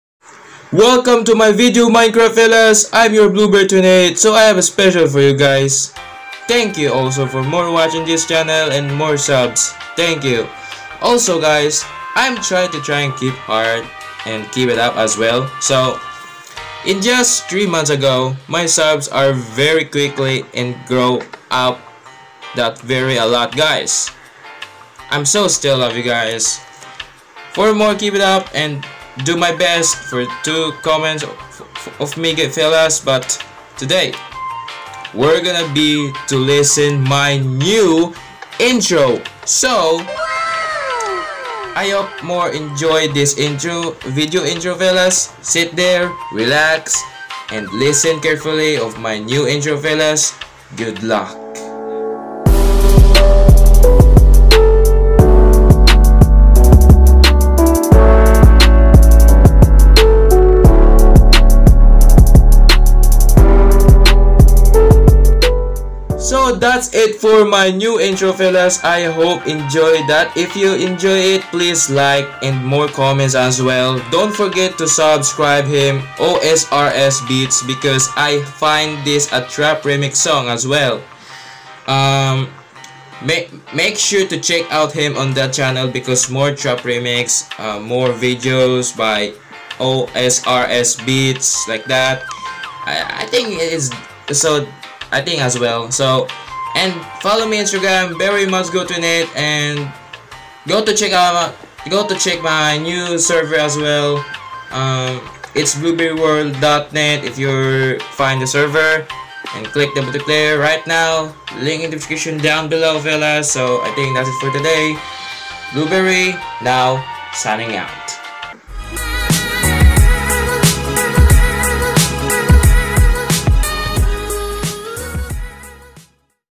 My New Intro fellas | for my gaming channel
Hello there fellas🙂

Welcome to my gaming channel blueberry 28 a few past 2 weeks or 3 weeks im not upload my latest video So today were gonna be listen my new INTRO AT BLUEBERRY 28

I create this because my Subs are very near at 100 subscribers so we celebrate that and i start my first pranking videos in minecraft😁👊🔥

AND
Make sure to check out as well in played on

Plssss listen my intro if you love it then like
NOW ! !

Edit by: 3.0 liscene plated contack
Made kinemaster